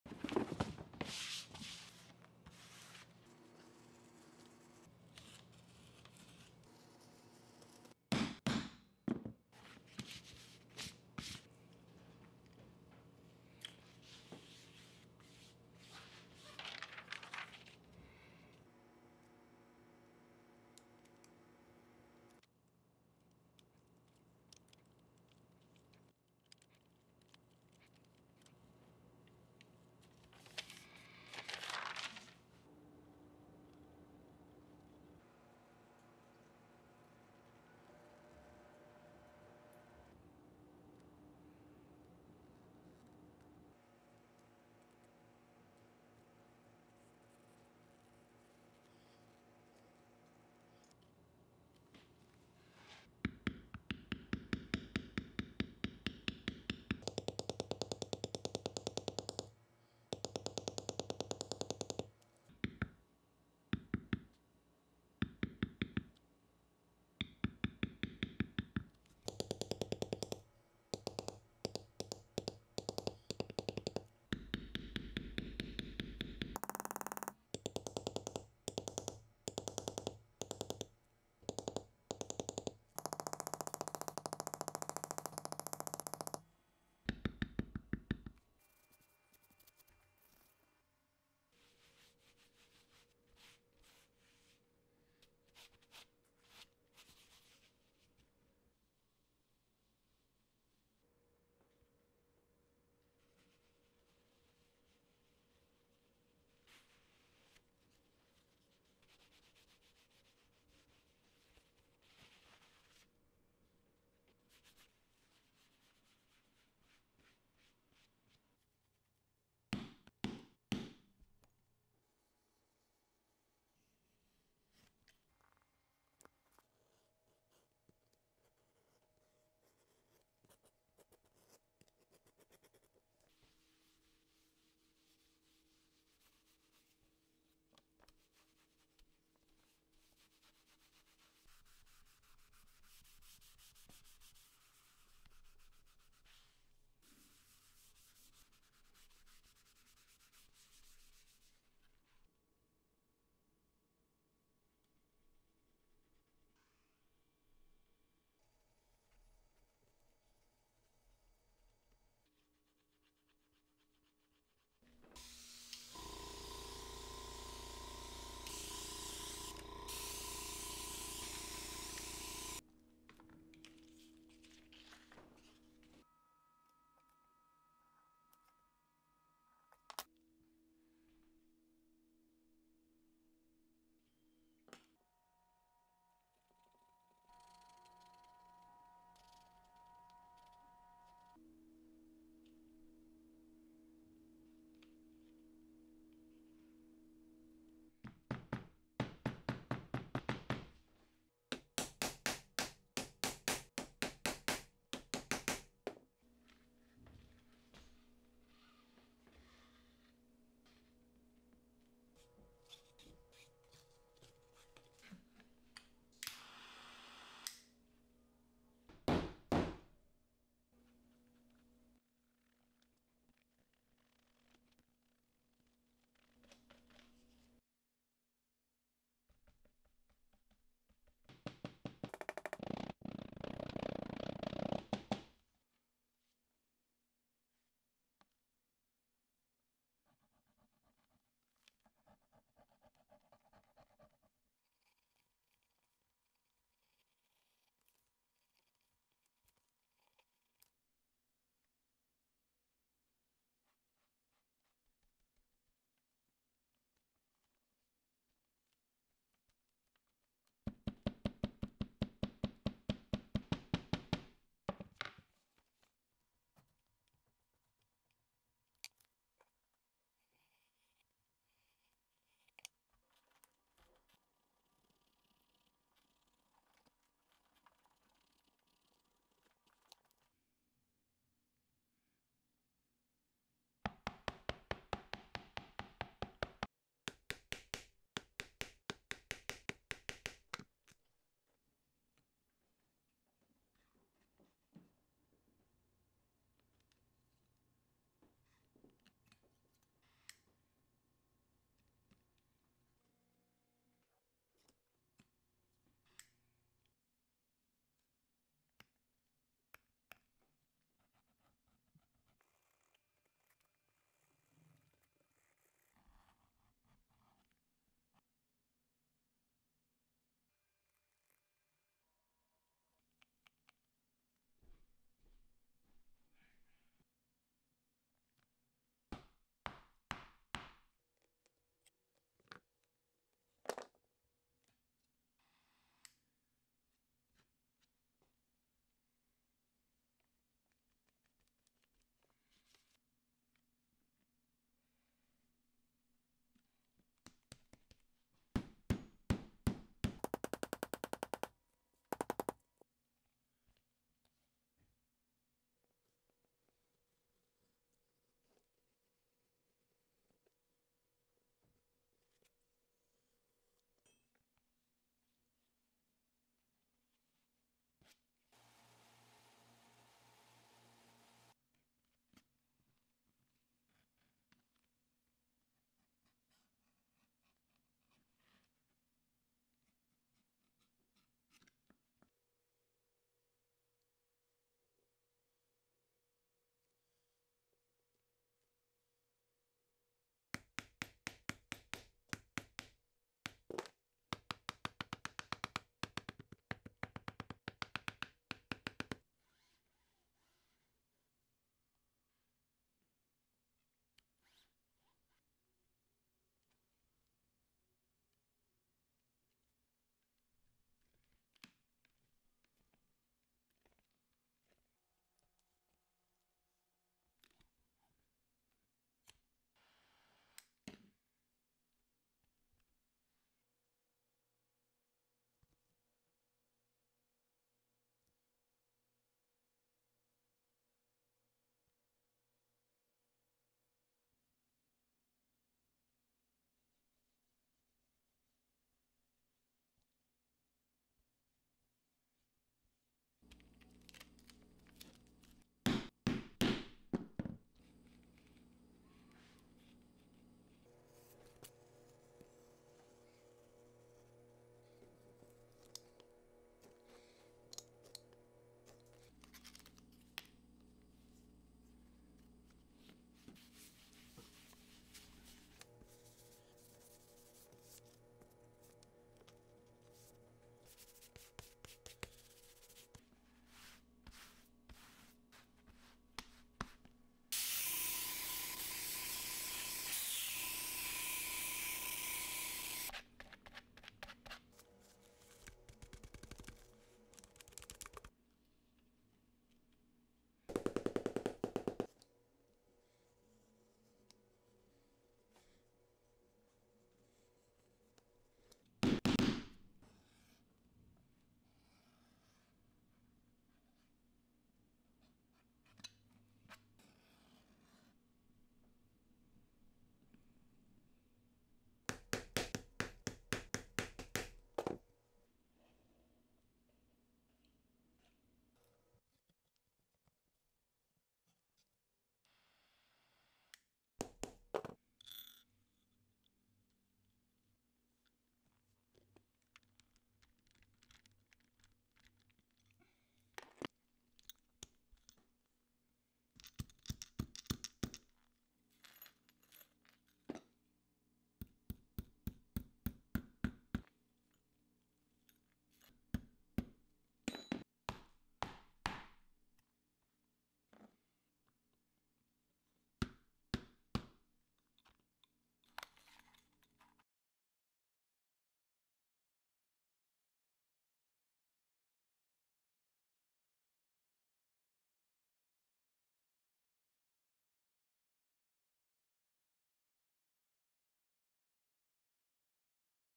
Making a BIKER WALLET - ASMR Style
Making a Biker Wallet with no talking, no music, and only tool sounds, giving it that ASMR vibe if that's your thing. Or at least not having me ramble over it. I do love leather carving, so I've put a Viking Style Odin God Mask on this one.

Get the Pattern
 - shorturl.at/cmqtM

Get the Wallet
 - shorturl.at/bgm24

  // Your support helps me do what I do //

🔶 *TOOLS & SUPPLIES*
● Ultra Deluxe Tool Kit